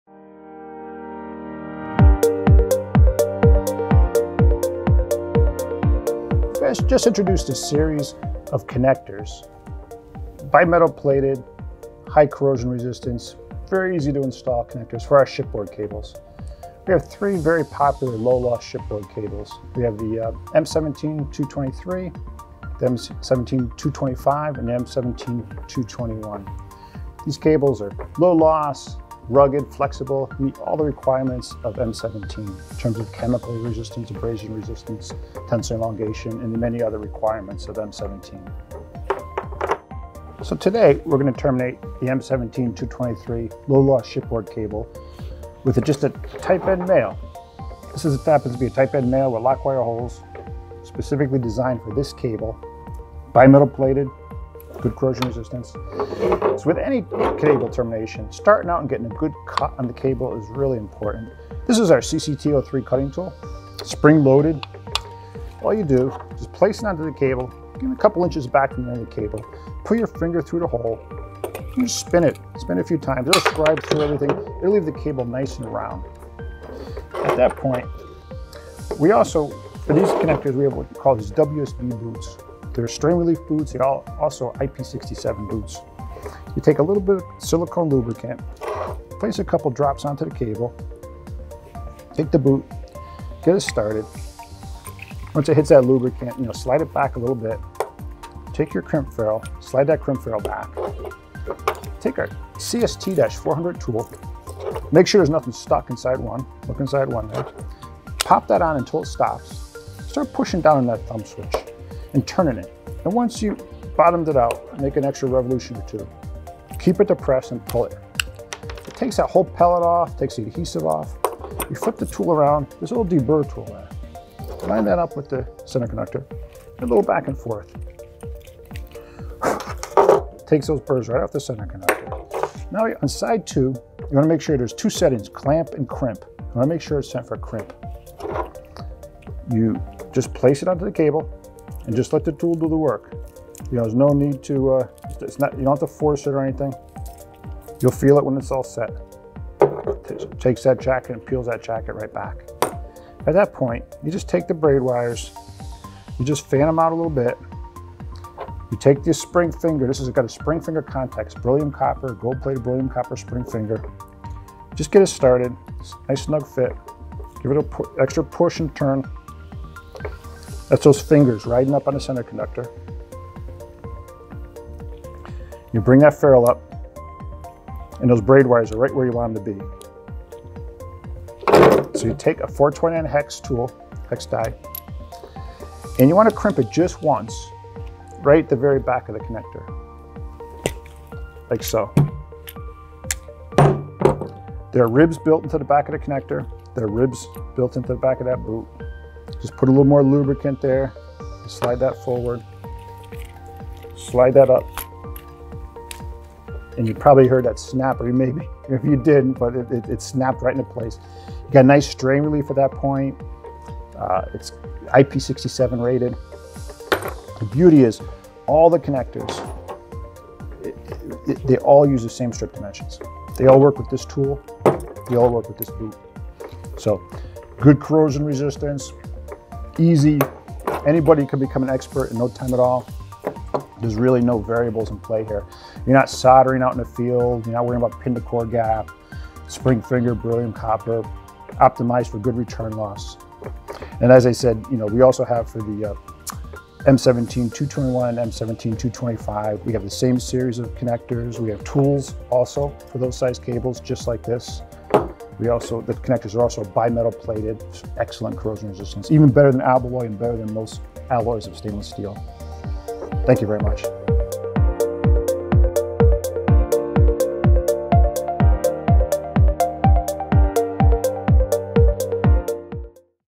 Demo Discoveries: Perfecting Shipboard Cable Termination with Times Microwave Systems
In this video, our Product Manager, Kevin Moyher, demonstrates how to terminate the M17/223 shipboard cable in an easy-to-execute way while using Times cutting, prepping, and crimping tools. The CCT-03 cutting tool provides a clean, square cut, while the CST-400 preps the cable for termination. Times Microwave Systems’ popular shipboard cables are rugged, low-loss, flexible, and they meet the many requirements of M-17.

LLSB-400: 43112
CST-400: 3192-004